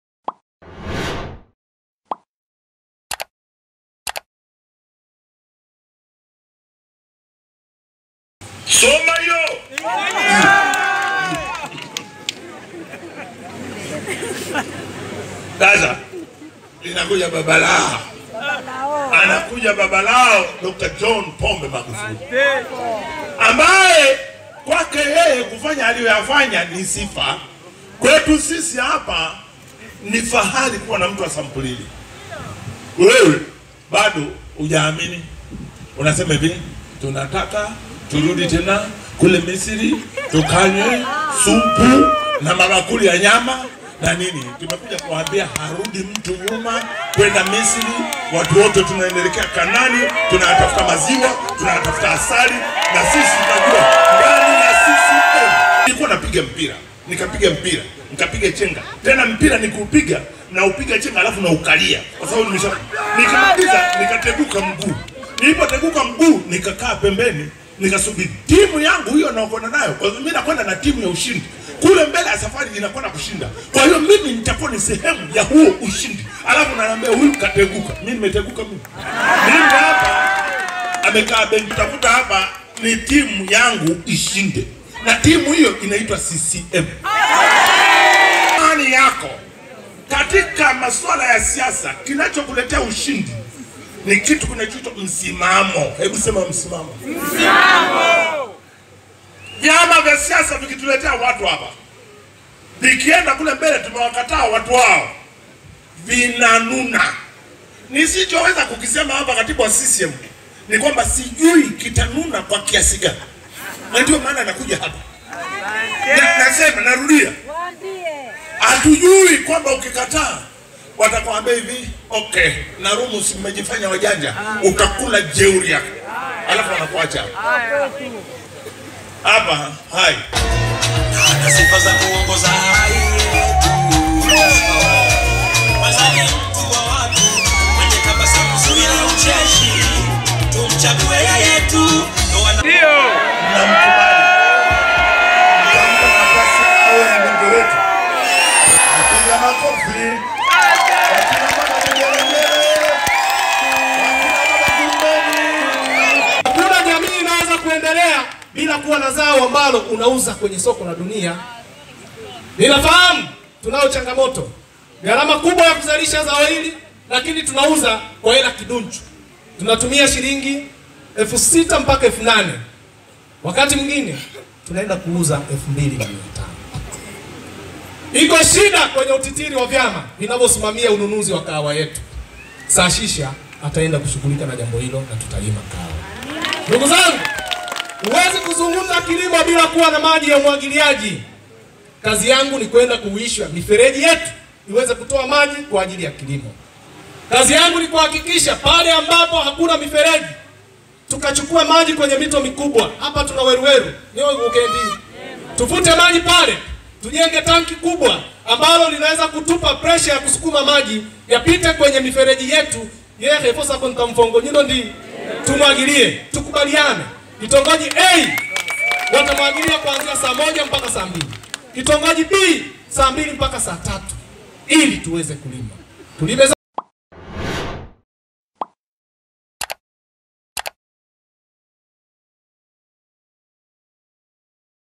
AGREY MWANRI AKIMNADI MGOMBEA UBUNGE JIMBO LA HAI
Tazama Mzee wa Toronto, Agrey Mwanri, akimnadi mgombea ubunge jimbo la Hai, kupitia CCM, Saashisha Mafuwe, na kumuombea kura Dk John Pombe Magufuli